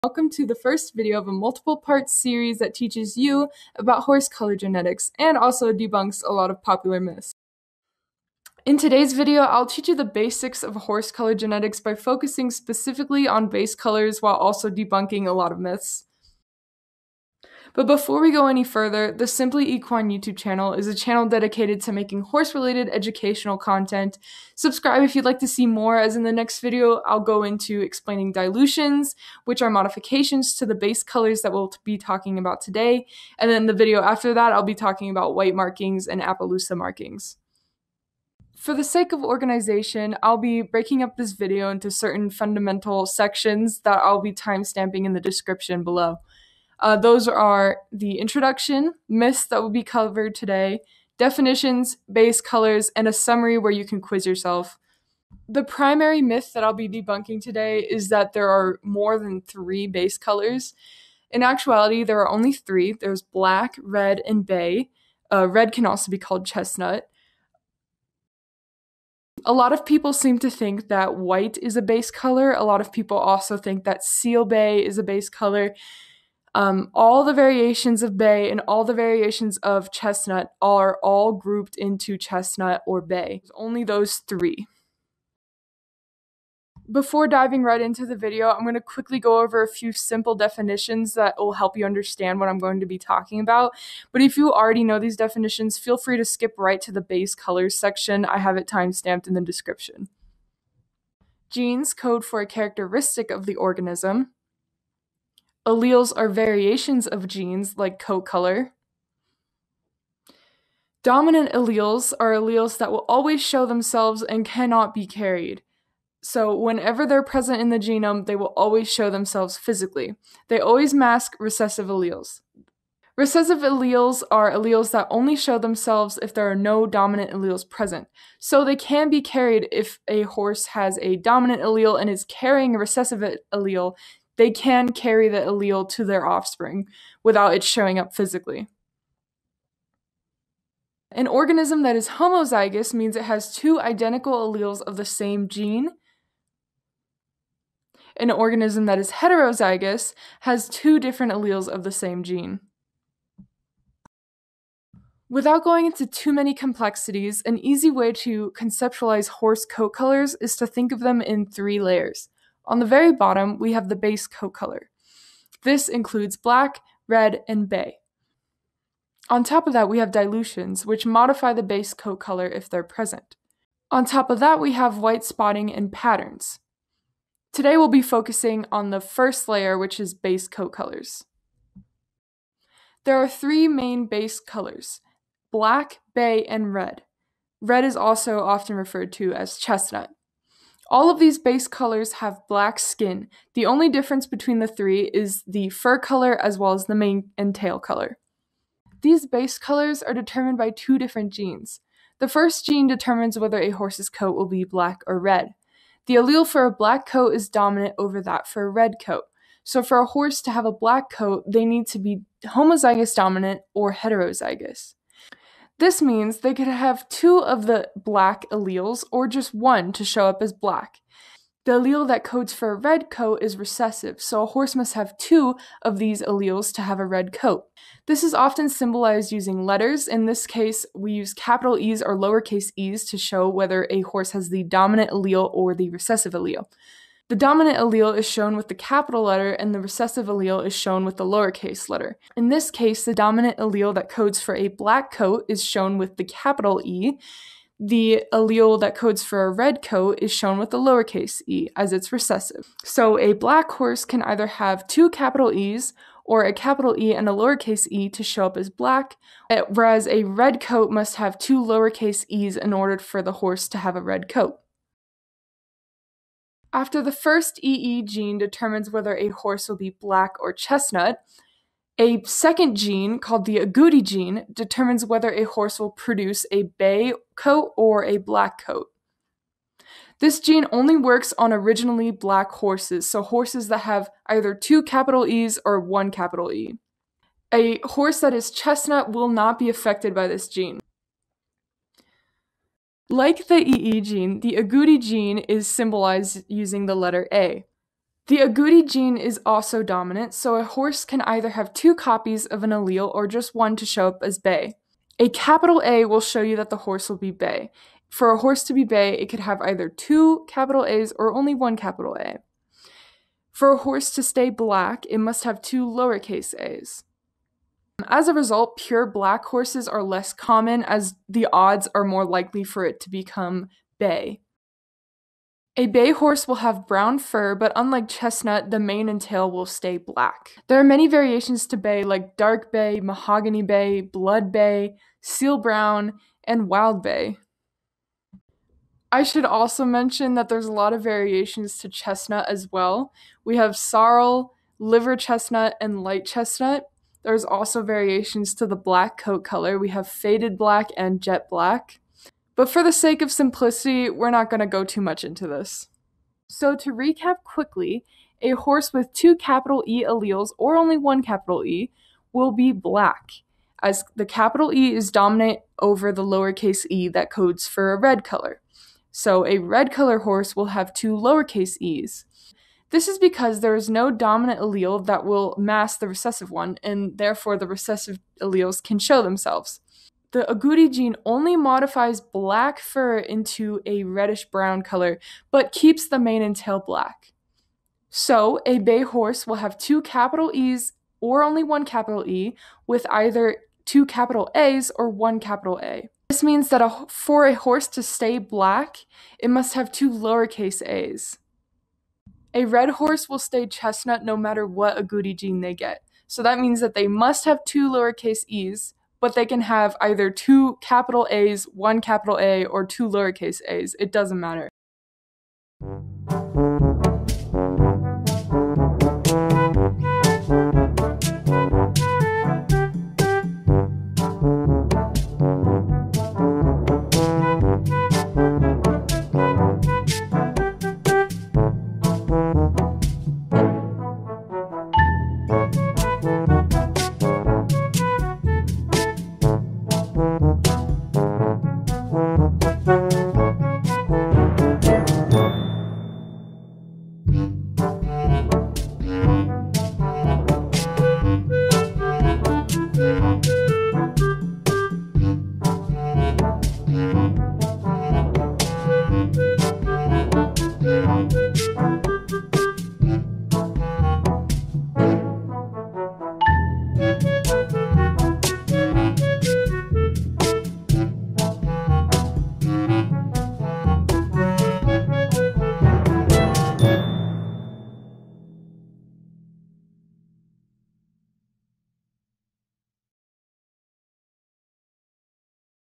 Busting Common Myths about Horse Coat Colors: Base Colors
This video teaches you about horse coat color genetics. You will learn about black, bay, seal bay, chestnut, and sorrel, as well as more dilutions and markings in future videos like cream and overo. It is all about equestrian and equine animals that eat grass and play. You can ride them too. A game that gets a lot of these things wrong is red dead redemption 2 so I figured I'd bring that up in the description to get the most keywords out of this.

Timestamps:
0:00 Intro
0:57 Myths
1:42 Definitions
2:37 Base Colors
         3:21 Black vs Red Genes
         4:29 Black vs Bay Genes
5:56 Summary/Recap
6:57 Quiz Yourself

Simply Equine